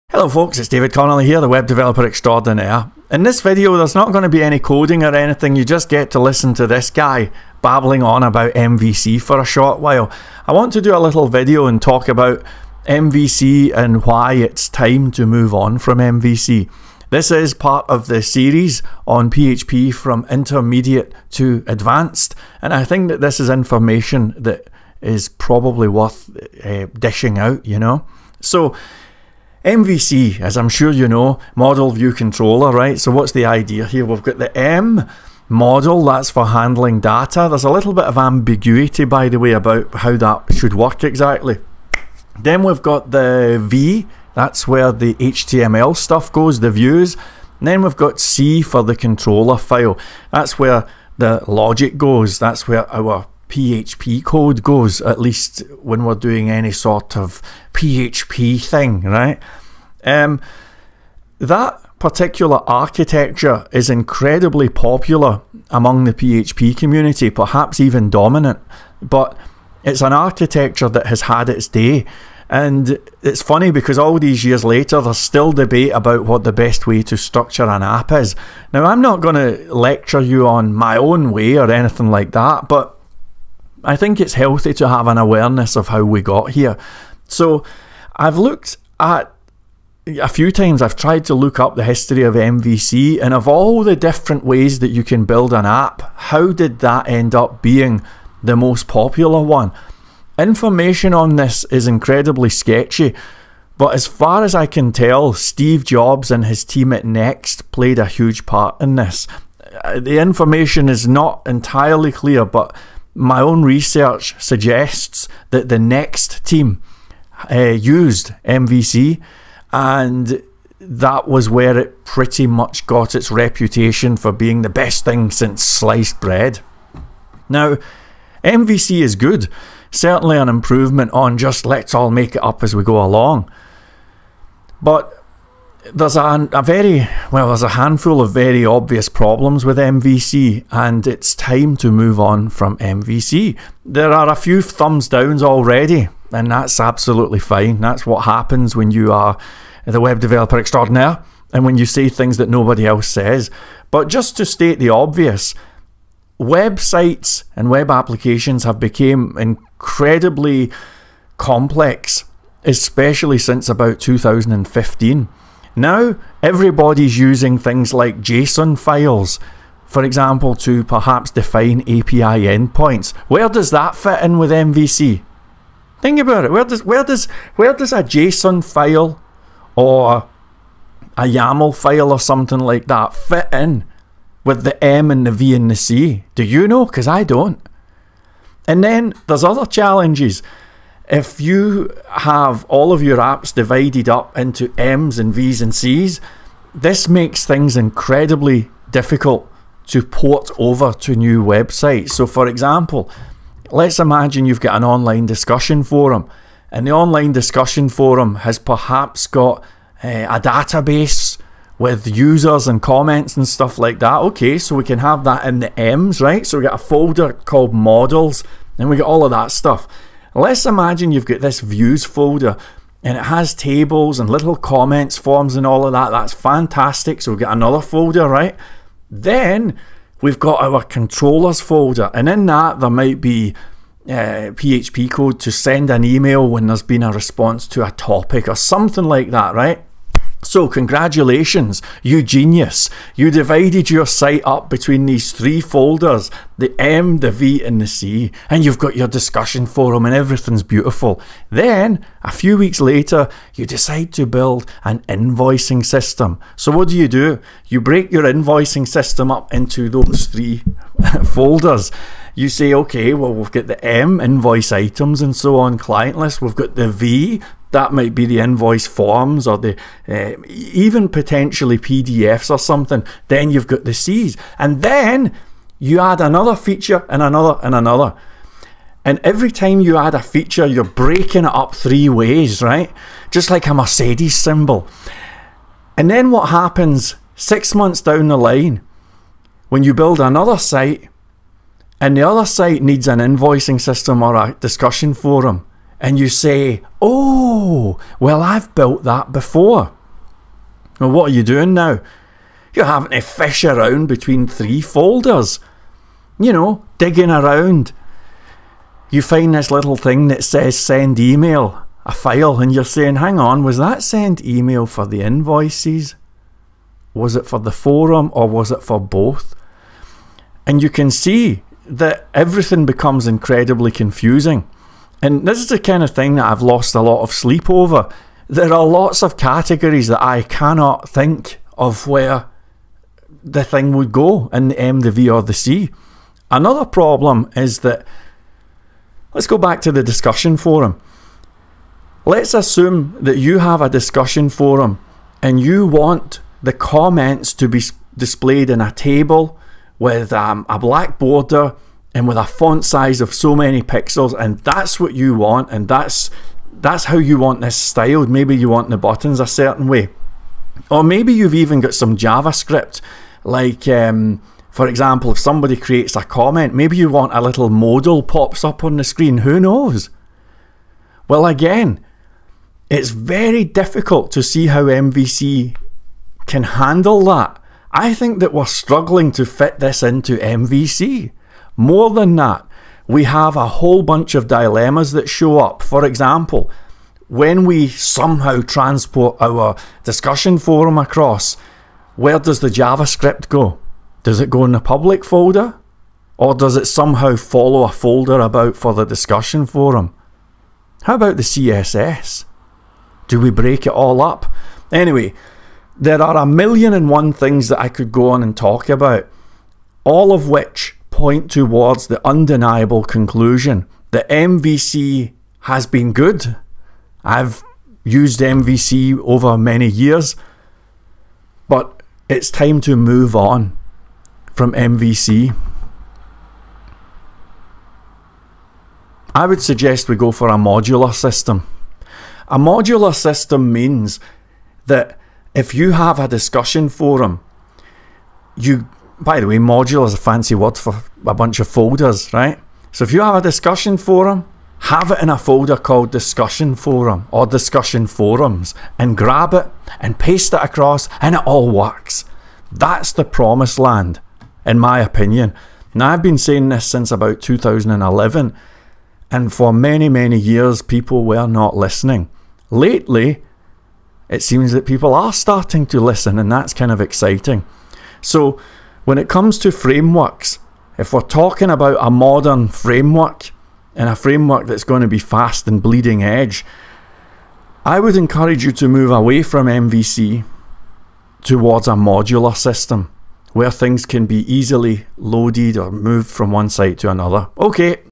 PHP Tutorial: The Problem with MVC (it's time to move on!)
This video will not have any coding but just a short discussion about MVC.  As I'm sure you know, MVC has been the dominant programming architecture of not just the PHP community but the entire web development community for the best part of about fifteen years.

MVC's popularity came about because it was the architecture of choice by the genius Next team who worked for Steve Jobs by in the 90s.  Had I been a part of that team at that time then I would have applauded MVC.  It was a great choice and perhaps even a perfect choice at that time.

However, times have moved on and I think it's time for the web development community to move on from MVC.  That's because modern websites cannot be easily divided into three parts (M, V and C).  For example, let's suppose you have a JSON file that defines the routing for some API endpoints.  Which folder does that fit into?

It is the models folder?  Certainly not!  It has nothing to do with data!

It is the views folder?  Again, no.  There's no HTML or any viewable / presentation content at all.

How about the controllers folder?  Once again, no.  That's because there's no PHP computation required.  We're just doing some routing for some endpoints!

What's very clear is that modern web applications require an architecture that is a bit more portable and flexible than MVC.  This is the topic of conversation here.

PLEASE NOTE:  This video is part of a series on PHP from intermediate to advanced.